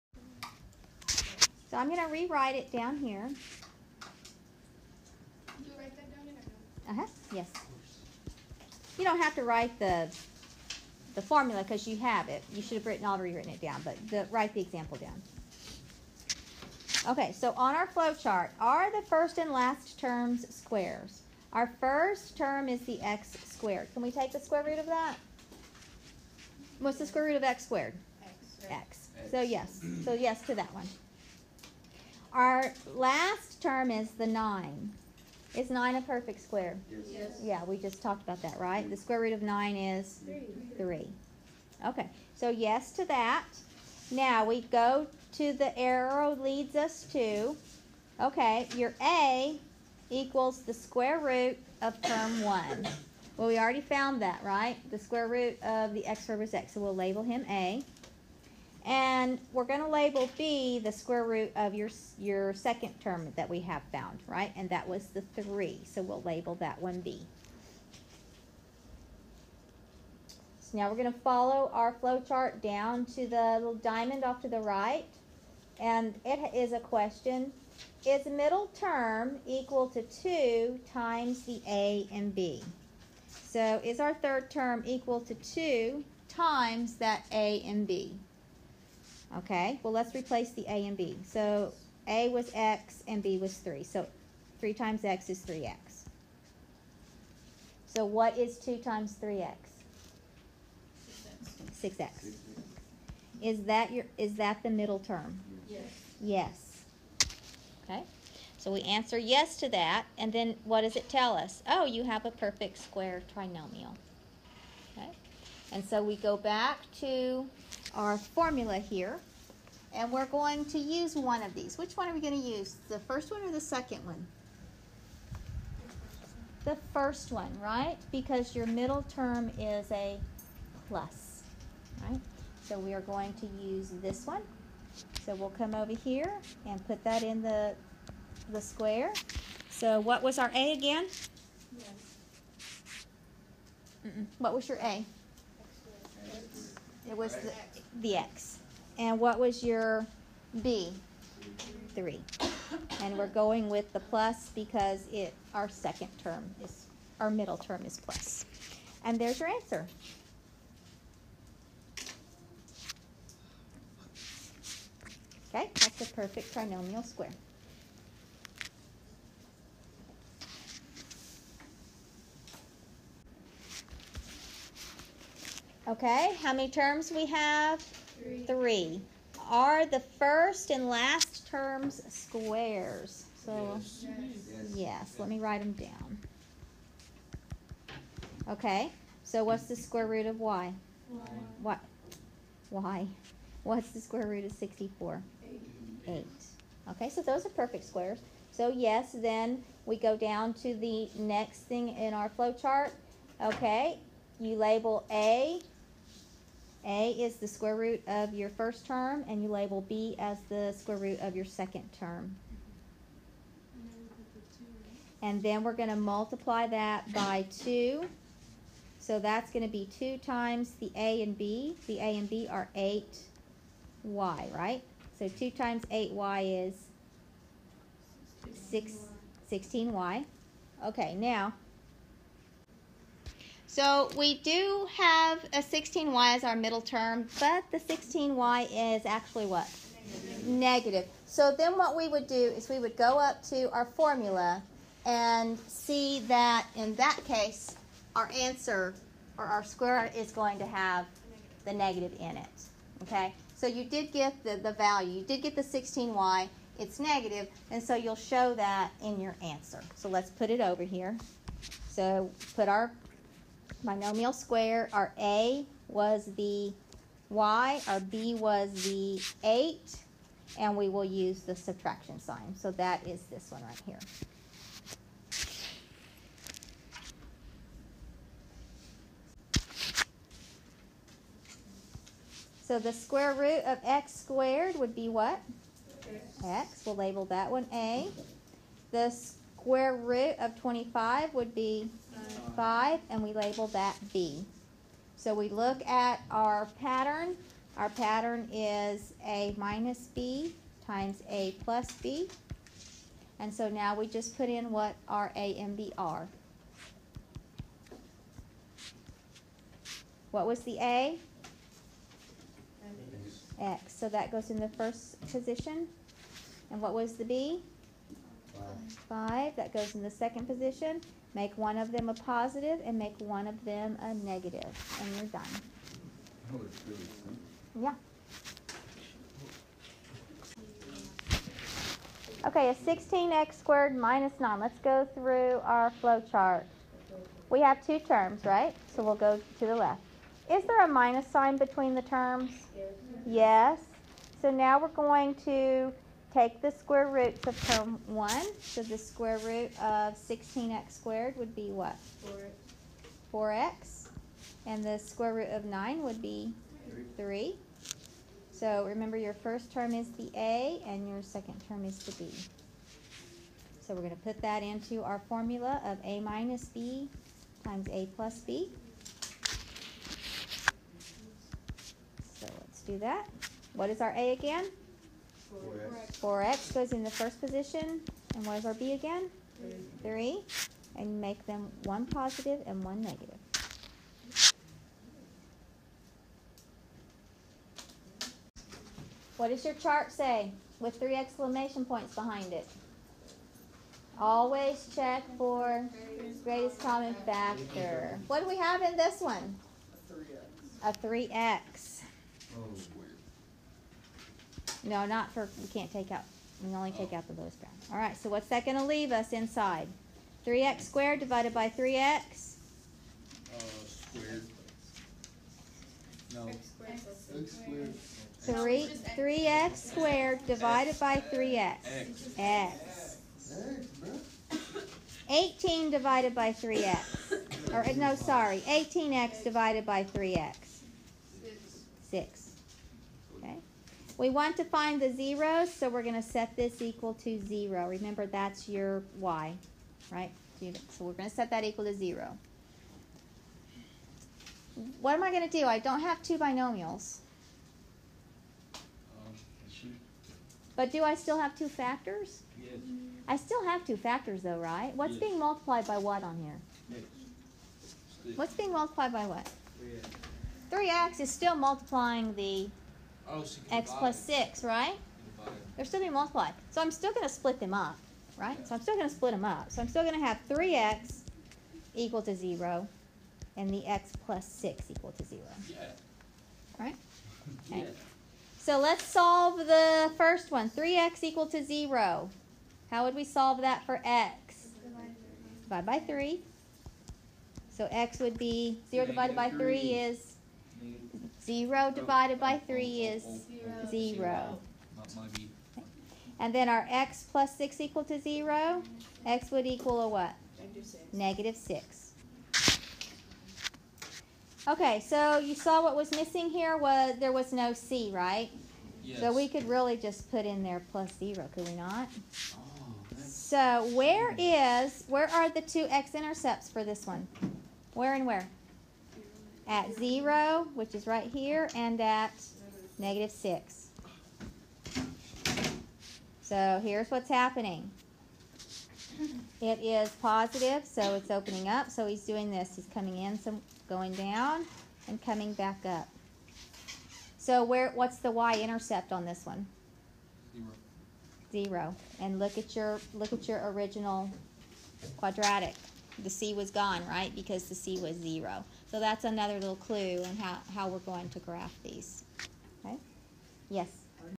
factoring special cases
Lecture on factoring special cases and solving quadratic equations